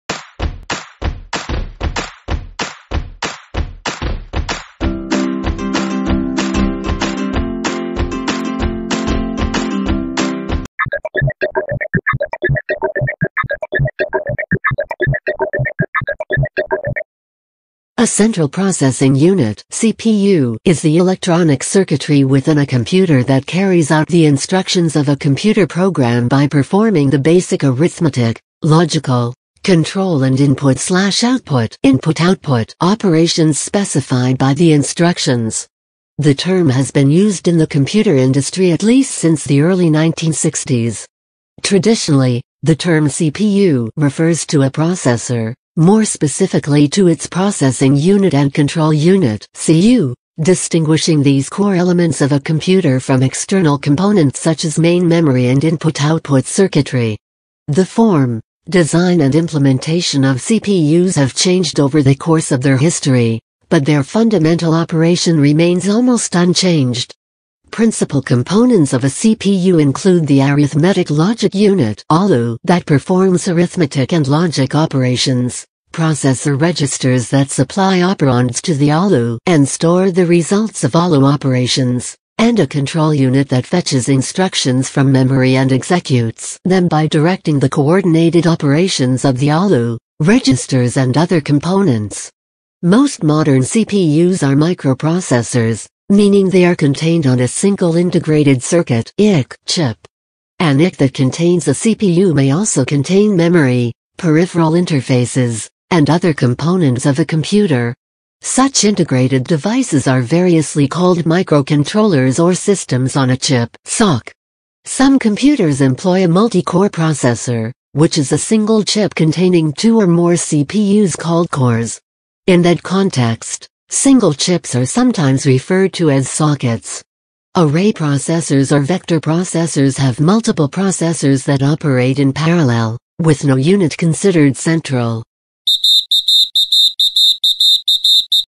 Your devices brain?? What a CPU is and how it works.
[credit audiopedia] Like us our electronics have a brain, so to speak. The CPU, a piece of monumental tech. Without this amazing technology, we could not use our everyday tech. As this video breaks down and explains the science and technology. In a way our whole lives are possible thanks to this innovative technology. Well at least for us in the more technical parts of the world. Such a small and very often unseen device, is such a piece of life changing technology. And this unsung hero is just another reason why you just gotta love technology.